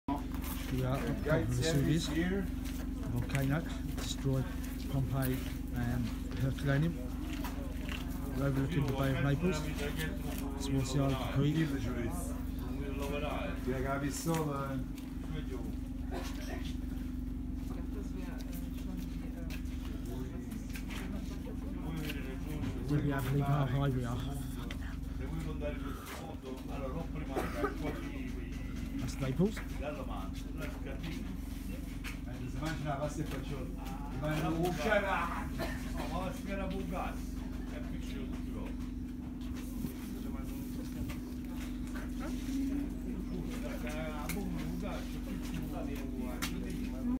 vesuvius
the view from the crater of the volcano overlooking the bay of Naples